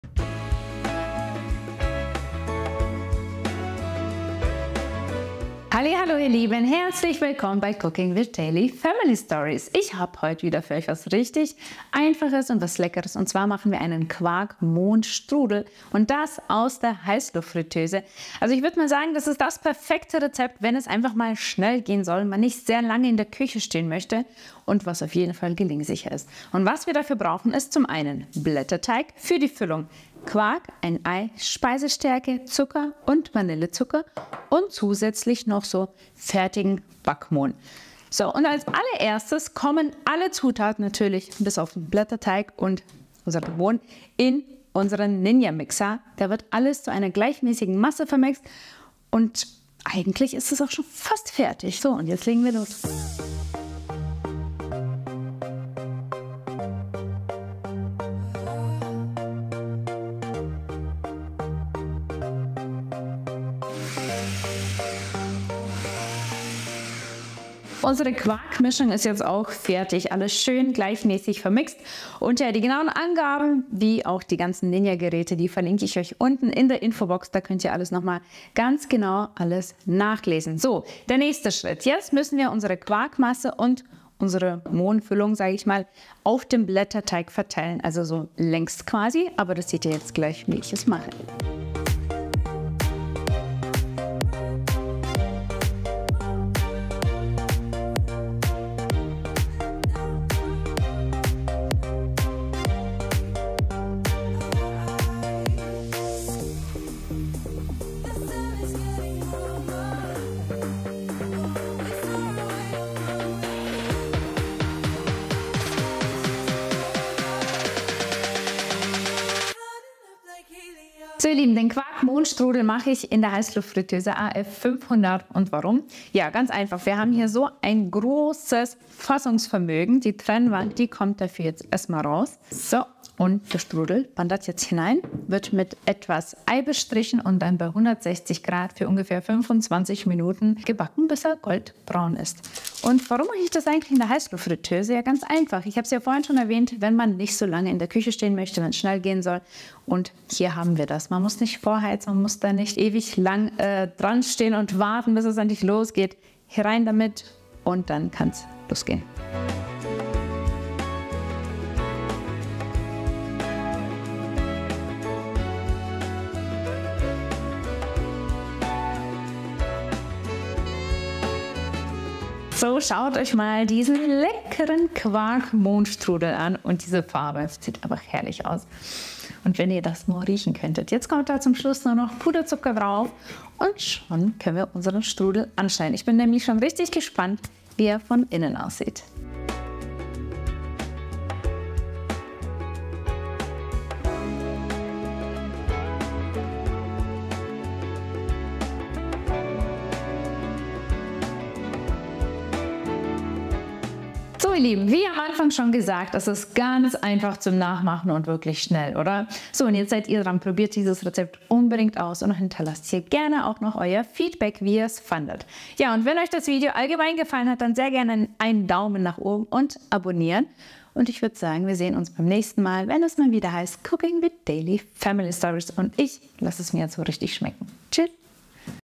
Saftiger Mohn-Quark Strudel aus der Ninja Foodi FlexDrawer Heißluftfritteuse! Einfach köstlich!
Entdecke mit uns die Magie der Ninja Foodi FlexDrawer Heißluftfritteuse! In diesem Video teilen wir das ultimative Rezept für einen unwiderstehlichen Mohn-Quark Strudel. Lass dich von der einfachen Zubereitung und dem knusprigen Ergebnis verführen. 🥧✨Heißluftfritteuse

Und nun zum Rezept!

Schneller Mohn-Quark-Strudel

Wir brauchen

-  1 Rolle Blätterteig
-  Backfertige Mohnfüllung

-  300g festen Quark (Tworog)
-  4 EL Zucker
-  1 EL Vanillezucker
-  2 EL Speisestärke
-  1 Ei

-  1 Ei (zum bestreichen)
-  Puderzucker

Und so einfach gehts

-  Für die Quarkfüllung alle Zutaten im Mixer zu einer gleichmäßigen Masse vermischen.
-  Blätterteig ausrollen, die fertige Mohnfüllung und die Quarkfüllung drauf verteilen und aufrollen.
-  Den Strudel mit einem verquirlten Ei bestreichen und in der Heißluftfritteuse bei 160 Grad, für ca. 10 Minuten und anschließenden weitere 15-20 Minuten, bei 140 Grad backen.
-  Zum Schluss mit etwas Puderzucker bestäuben